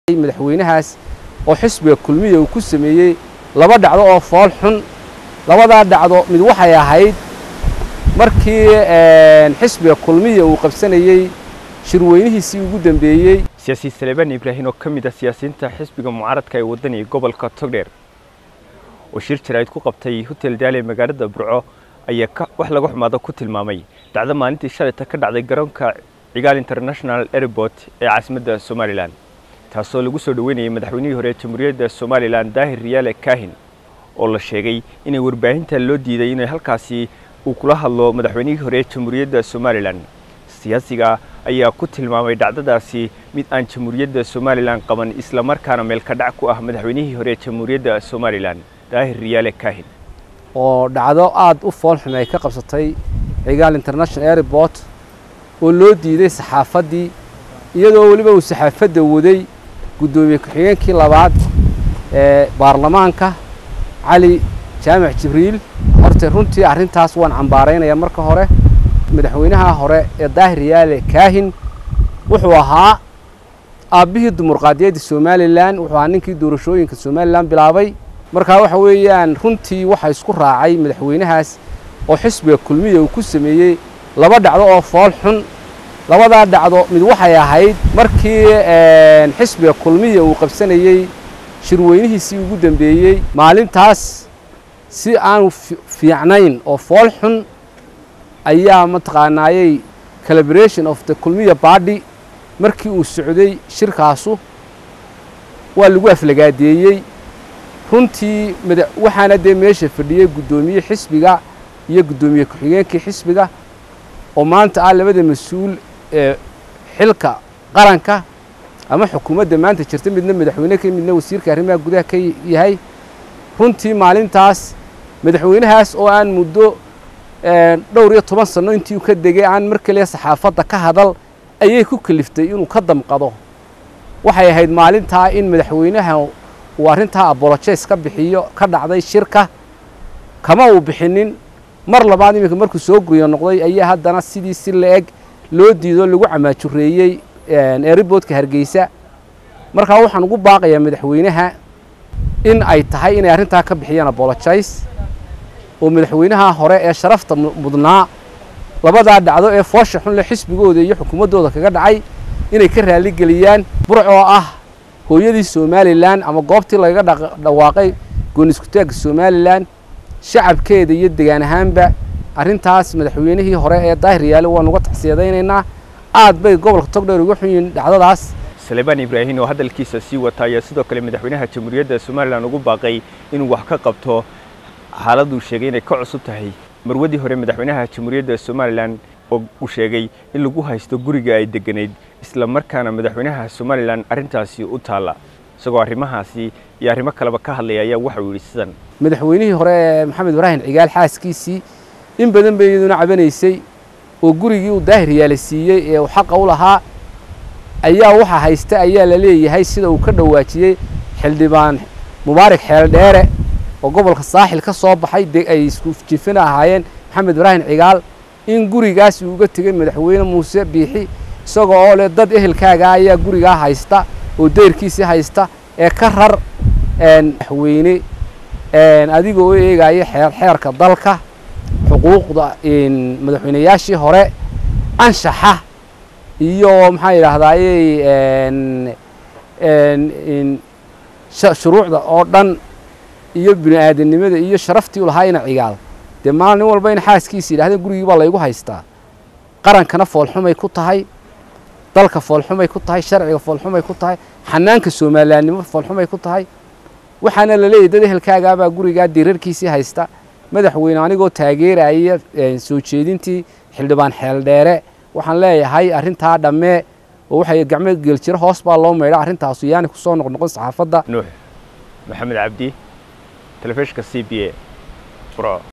"Xisbiga kulmiye Madaxweynihii Hore ee Somaliland Waxa Uu Kula Kacay Labo Dhaco oo Foolxun" Saleban
"Xisbiga kulmiye Madaxweynihii Hore ee Somaliland Waxa Uu Kula Kacay Labo Dhaco oo Foolxun" Siyaasi Saleebaan Ibraahim